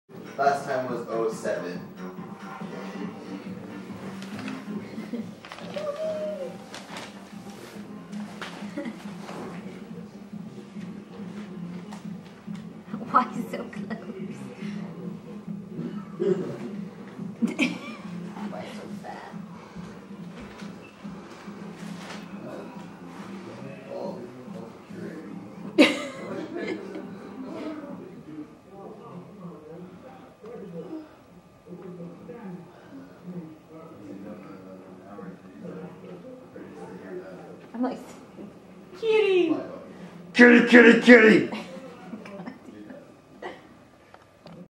Trophy gets pushed over
I push Trophy over and she doesn't care